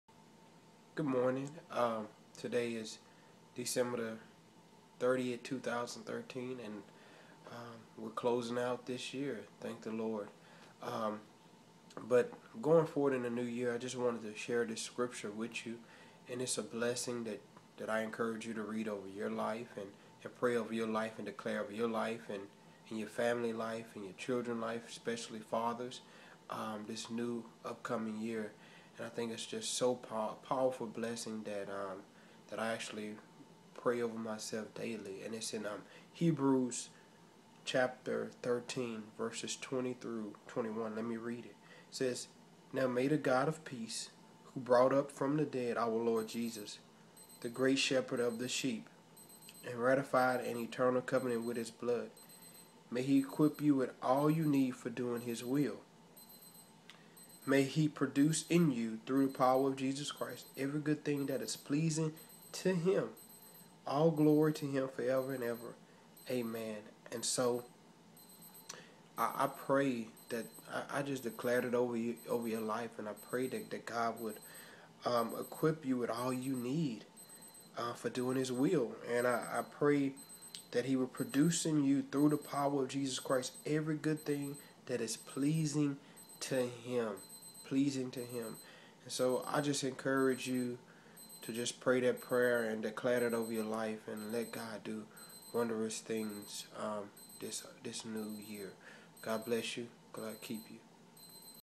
A Christian Blessing, Hebrews 13:20-21
The writer of Hebrews concluded their epistle by declaring a blessing over them. I pray that you would use this blessing over yourselves and others, especially Fathers, speak it over your children. Love you!